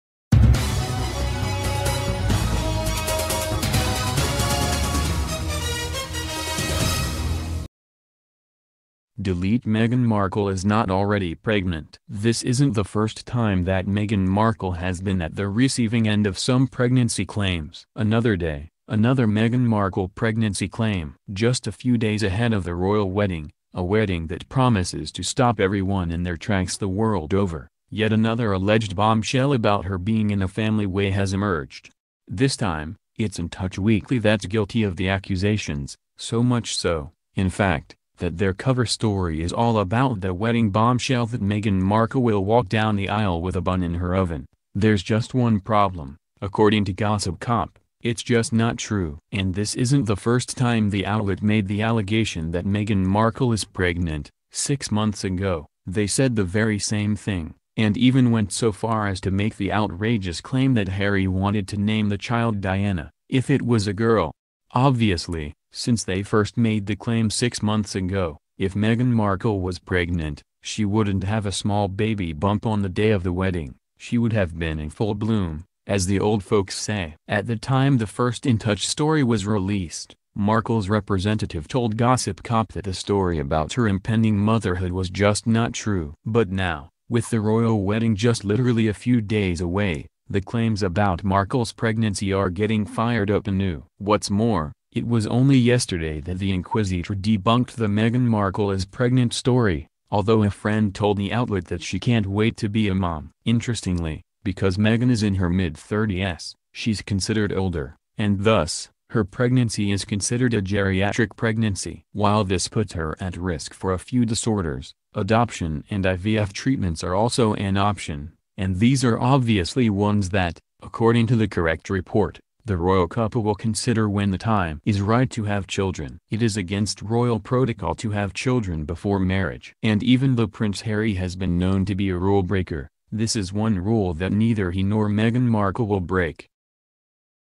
Wonderful child: Meghan Markle with small baby bump into royal wedding with Prince Harry
Meghan Markle Is Not Already Pregnant